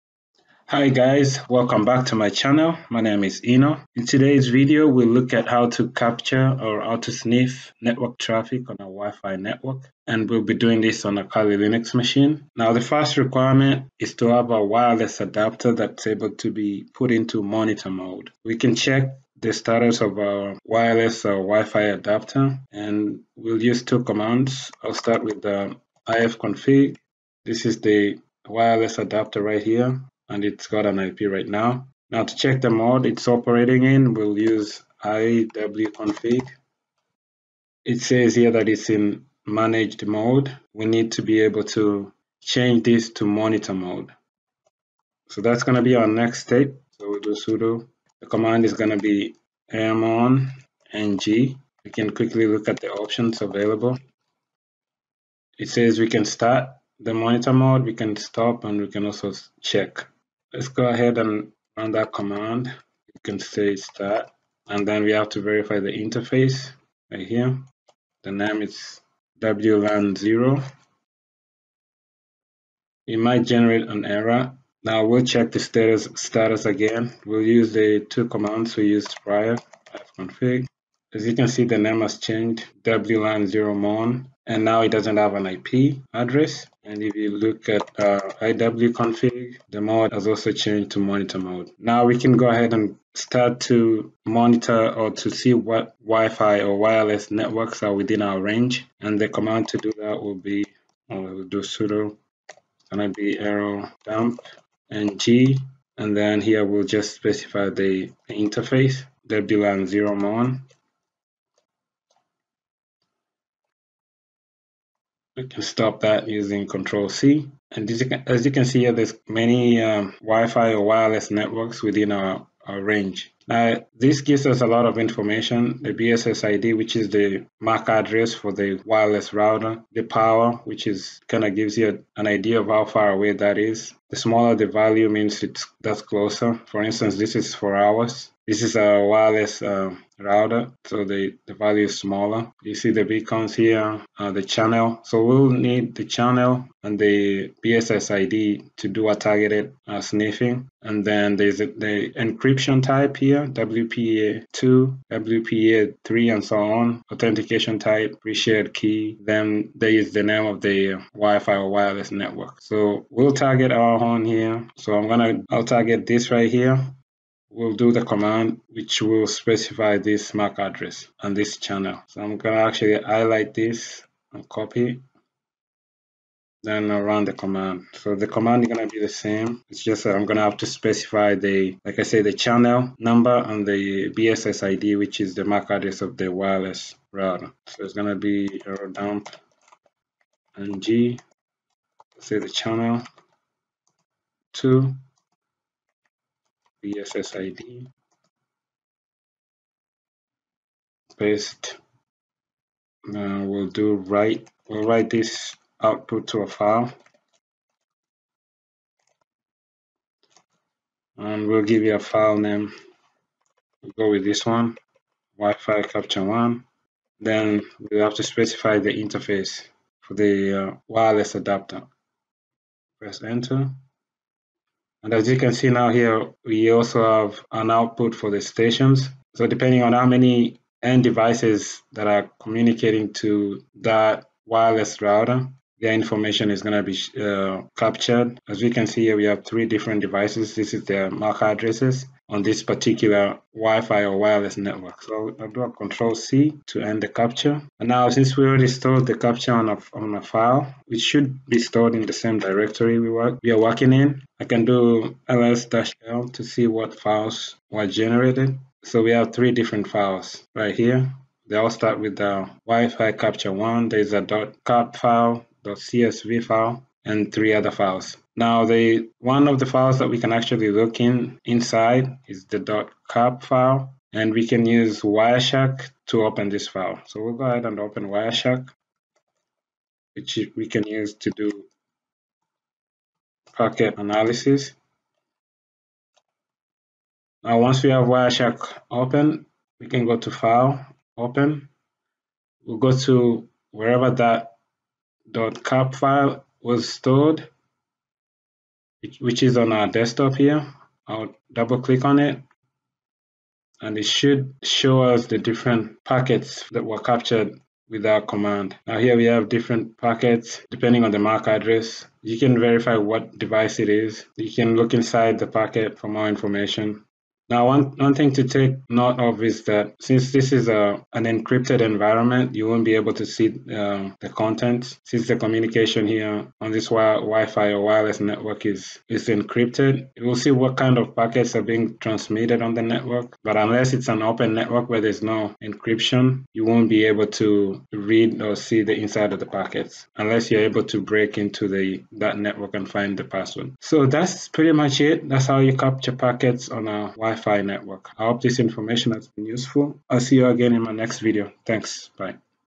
How to Sniff/Capture WiFi Traffic using Kali Linux
How to sniff/capture network traffic on a Wi-Fi/wireless network using Kali Linux. Penetration testing topic.